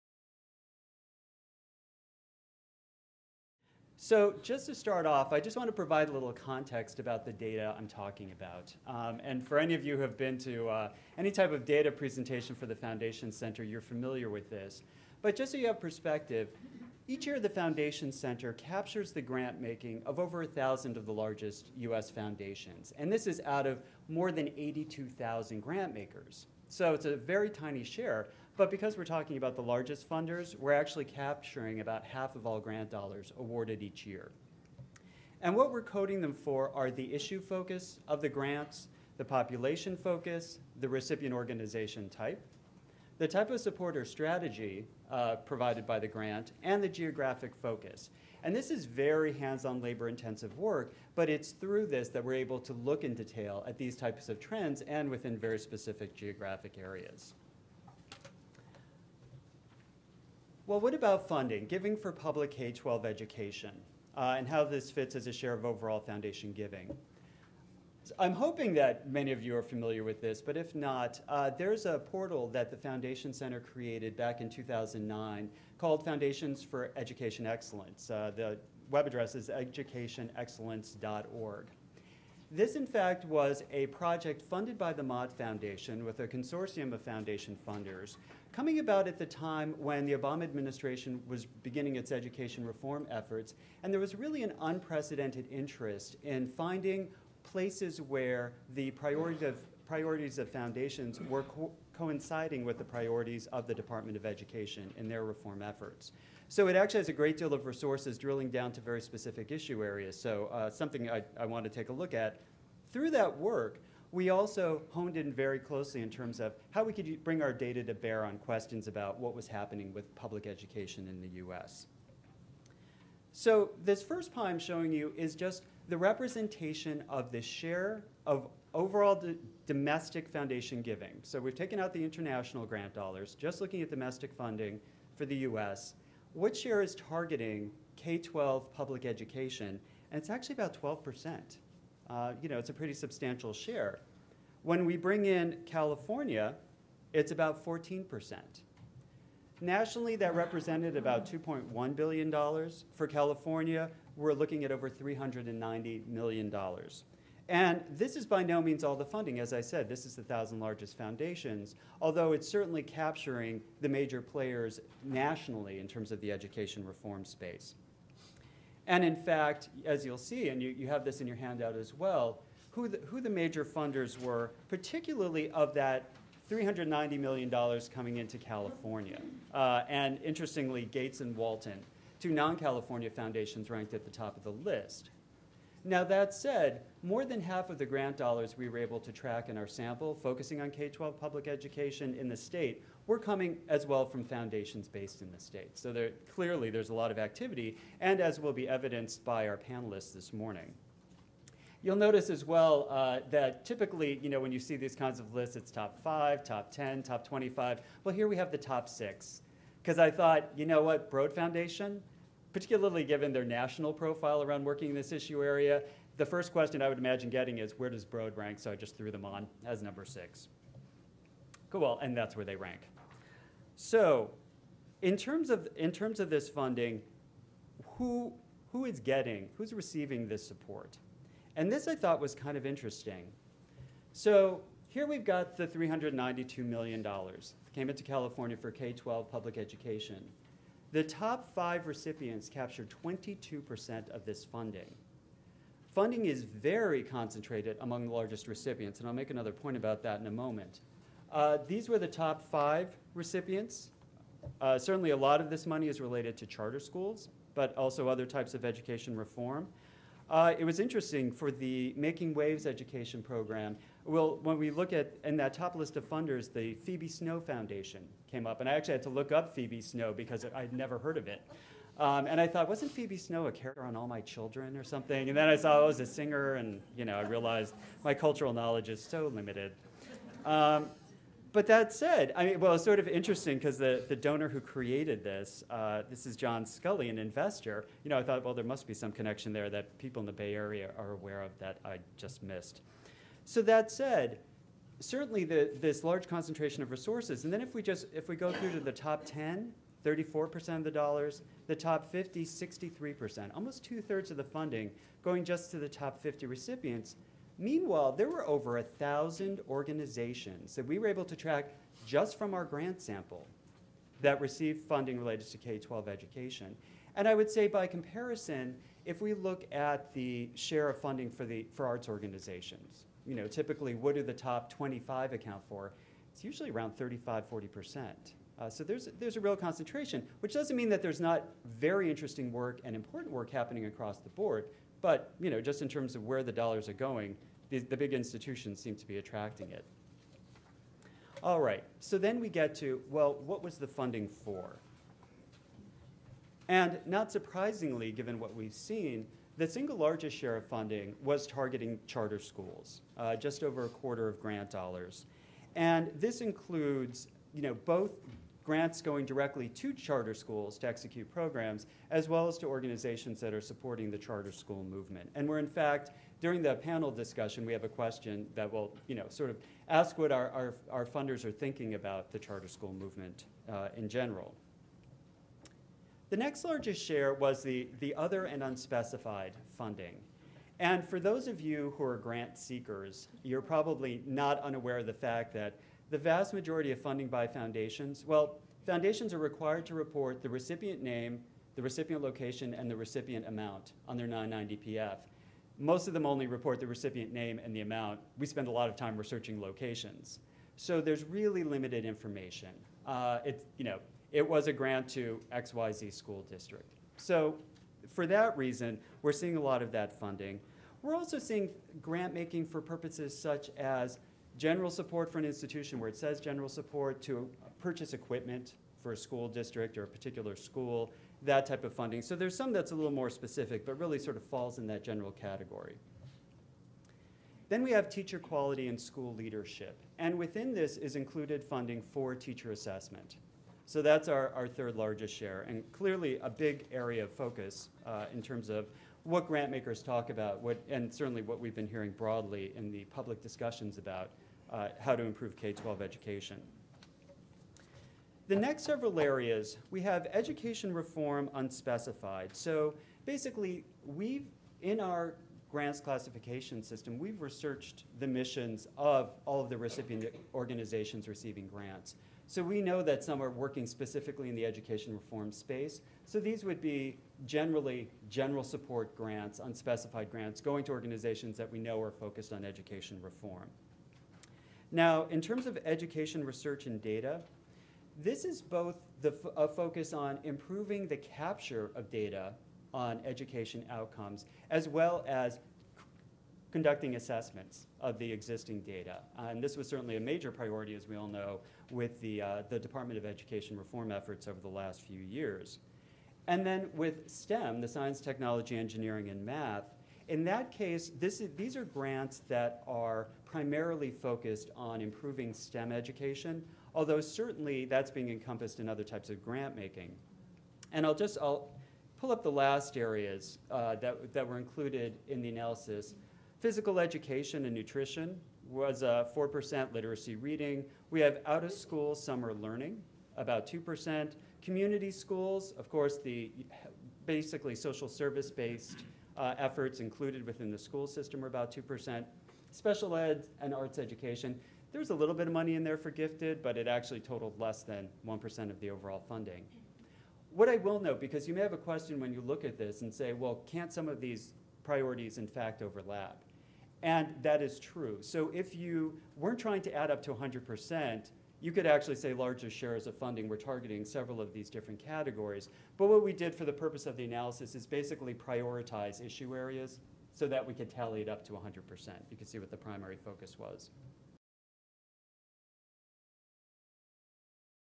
Racing to Improve K-12 Education How Are Foundations Helping to Shape the Future?
An overview of recent trends in foundation funding for public education both nationally and in California presented by Steven Lawrence, director of research, the Foundation Center.